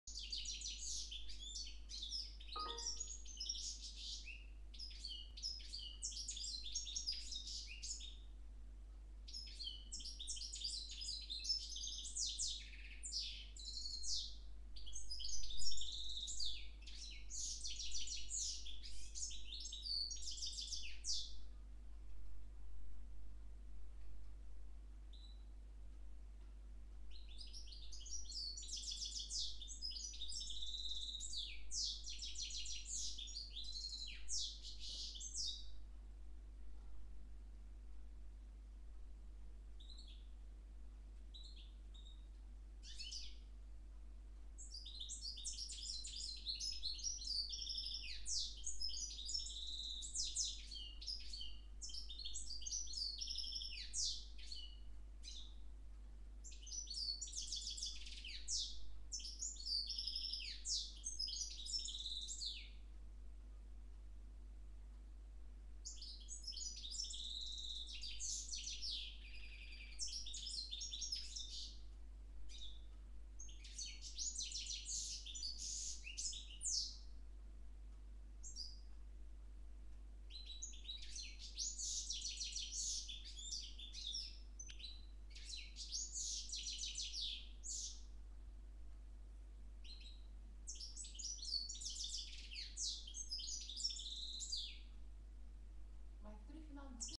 Chardonneret x timbrado : AbuAsfur's mulet 2010
AbuAsfur's mulet on 10 feb 2011 singing : parva x timbrado